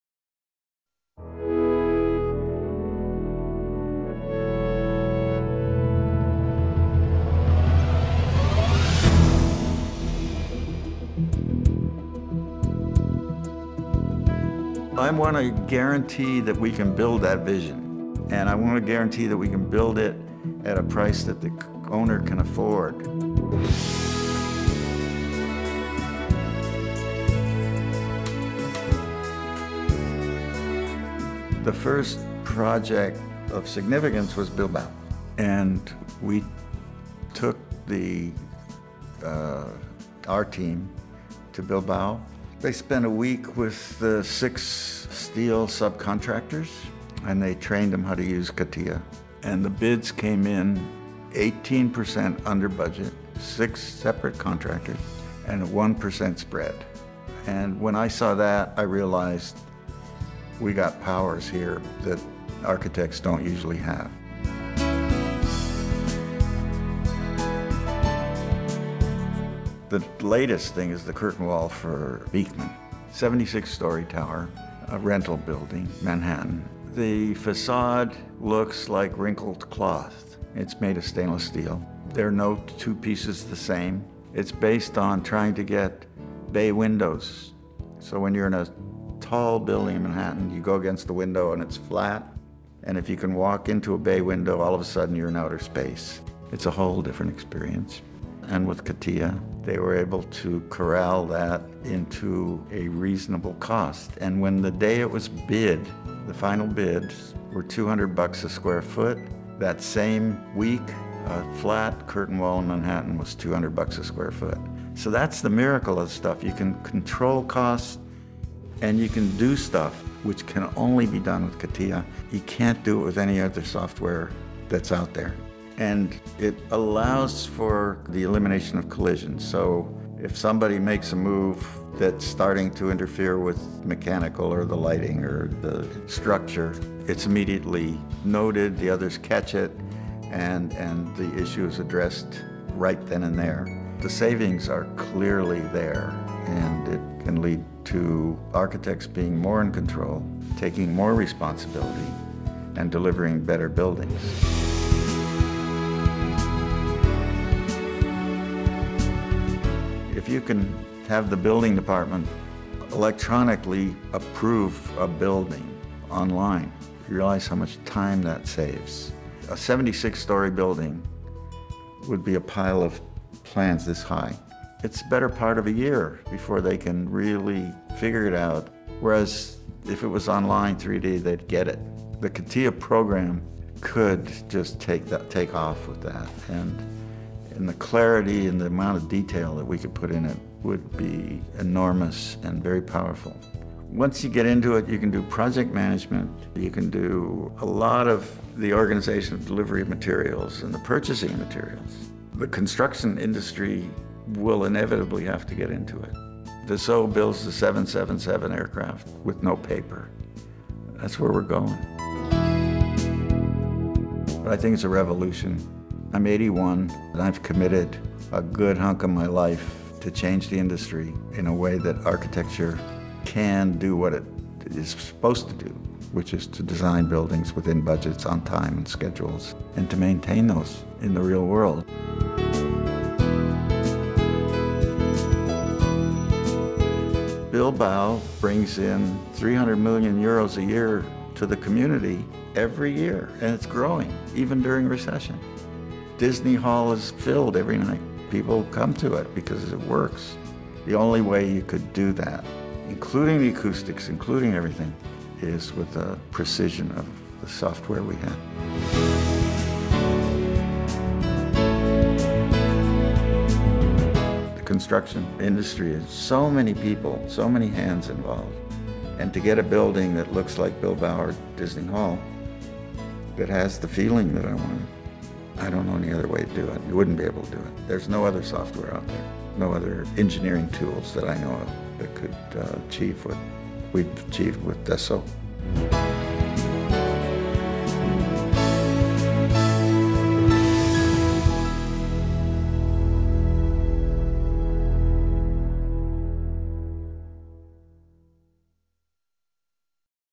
Frank Gehry uses CATIA for his architecture creations
Gehry talks how CATIA software is key to his architectural realizations.